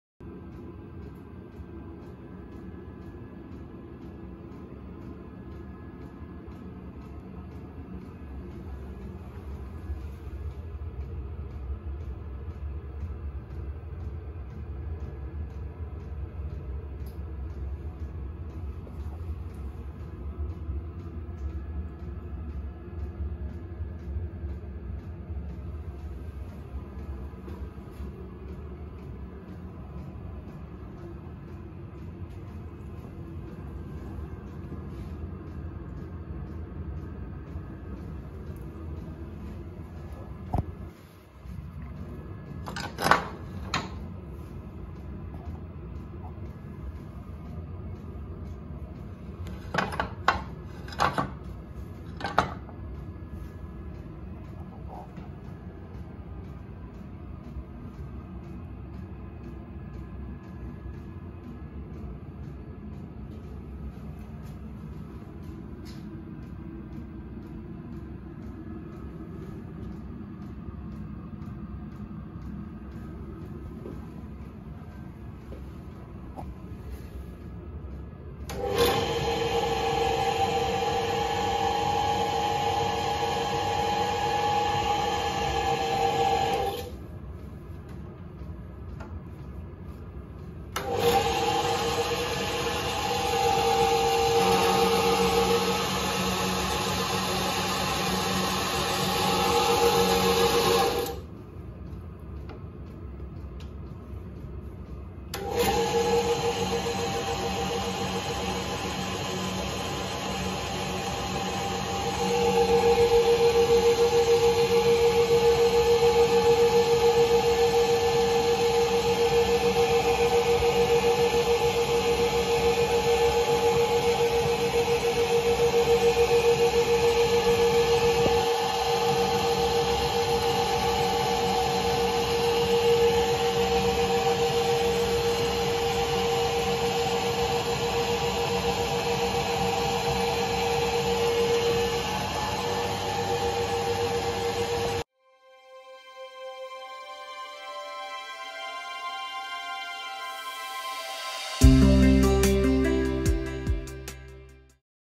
Myford ML7 Accessories Excellent Condition Lathe Drehmaschine Svarv Tornio Tour
Myford ML7

* Single phase 230V
* Very good overall condition
* Powerd feed
* Thread cutting
* Cutting dial
* Back gear
* Table included
* Lots of accessories
* MT2 tailstock
* Between centers: 500mm
* Center height: 90mm
* Spindel bore: 17mm

Follow the channel for more Quality Machine Tools!